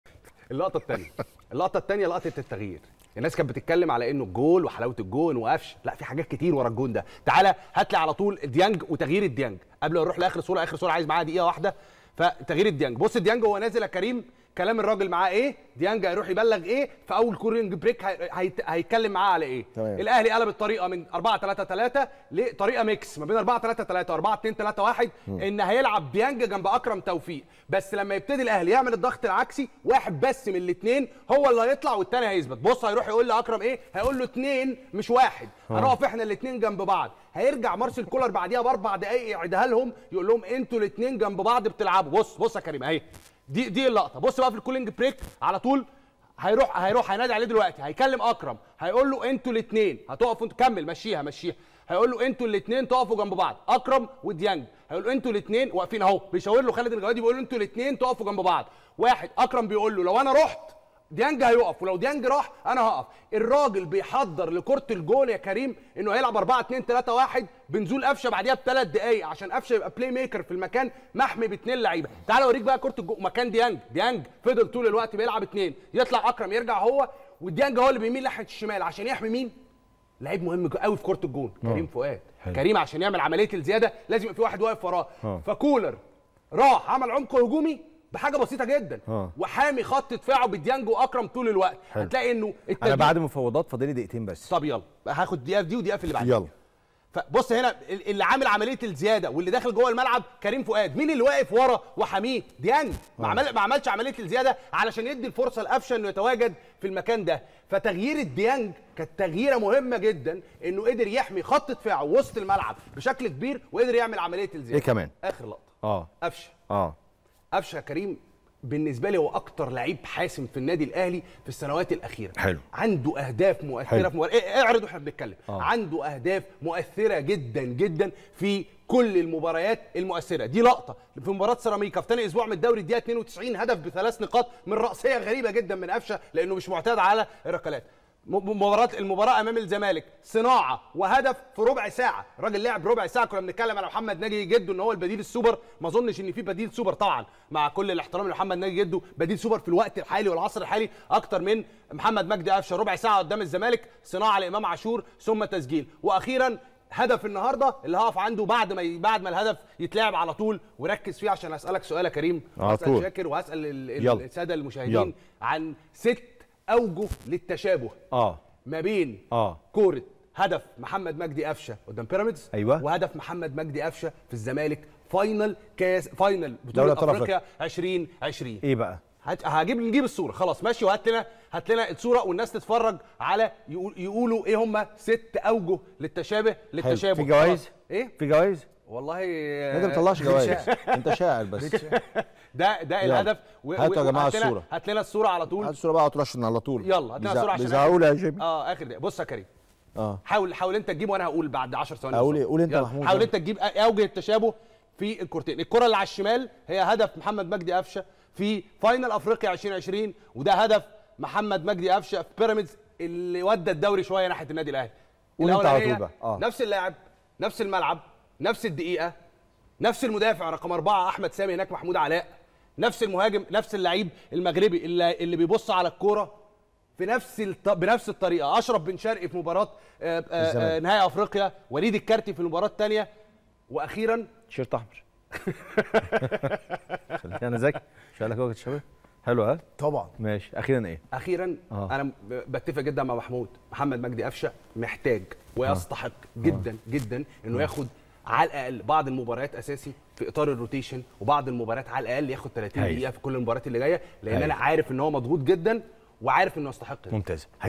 مقارنة بين هدف افشة في بيراميدز وهدفه في الزمالك في نهائي القرن "٦ أوجه للتشابه بين الهدفين"
محمد جمال هيعمل مقارنة بين هدف افشة في بيراميدز وهدفه في الزمالك في نهائي القرن "٦ أوجه للتشابه بين الهدفين" 😮🔥